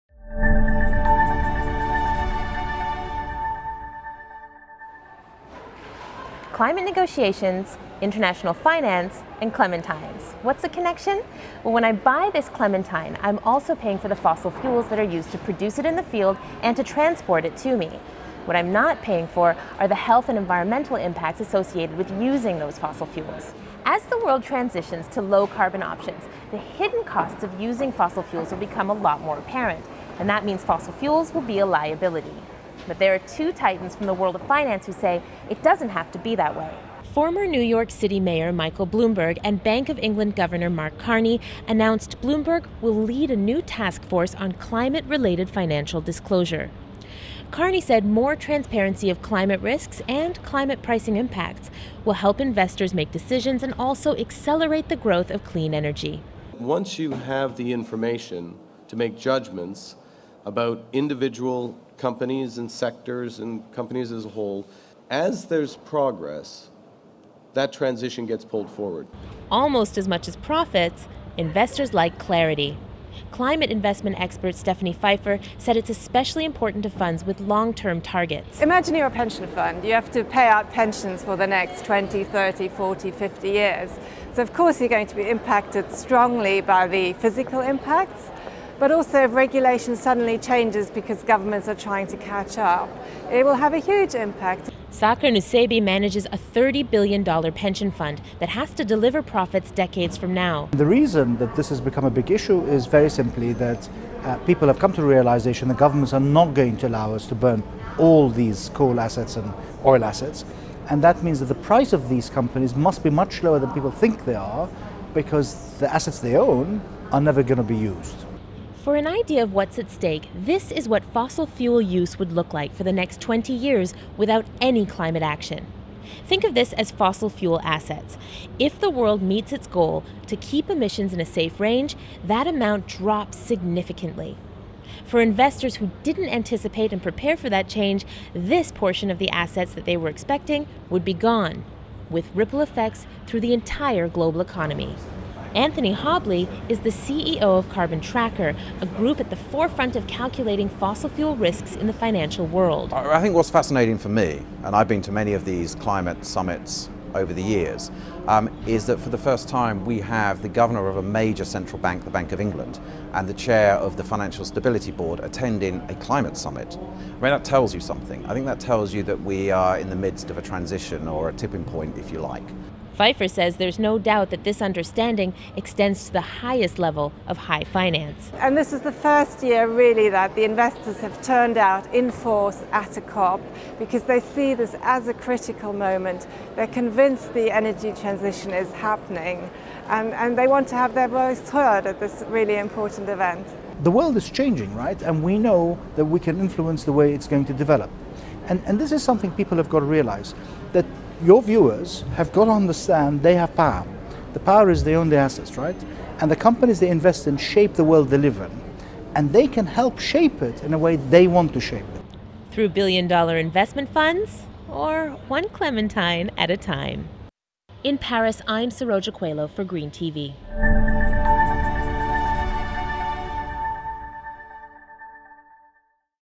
COP21 Daily News: Billionaires on the Scene at Paris Climate Conference
Financial sector wakes up to a world ready to tackle climate change.